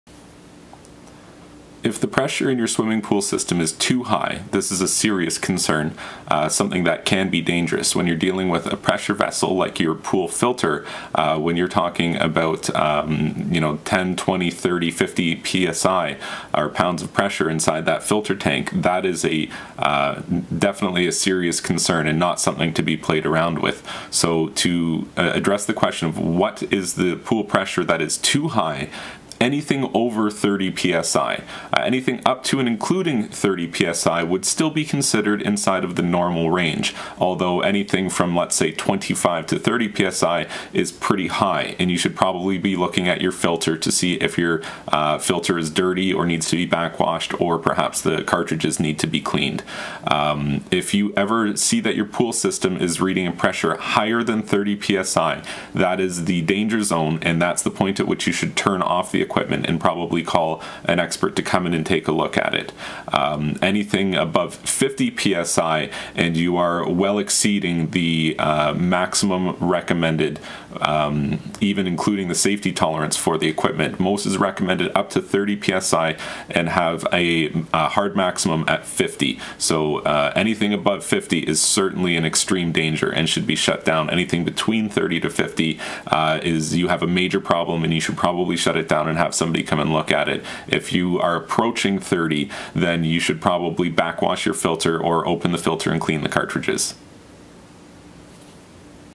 Pool Filter Pressure Too High
- Learn what pressure your swimming pool system should operate at and how much pressure is considered too much.

Swimming pool systems can be dangerous due to the pressures in the filter tank most specifically.  Pool equipment is typically designed to operate up to 30 PSI under normal operating conditions.  50 PSI is the hard maximum that pool equipment can handle before failure is likely to happen.  If you have a pool system that is operating over 30 PSI you should turn off the system and clean your filter (clean cartridge filters / backwash sand filter) to see if this helps the pressure to drop back below 30.  If the pressure stays over 30 PSI you should call a pool technician to inspect your equipment for you.

Watch this video of Steve reviewing a pool equipment installation and watch his reaction when he notices the pool filter pressure is too high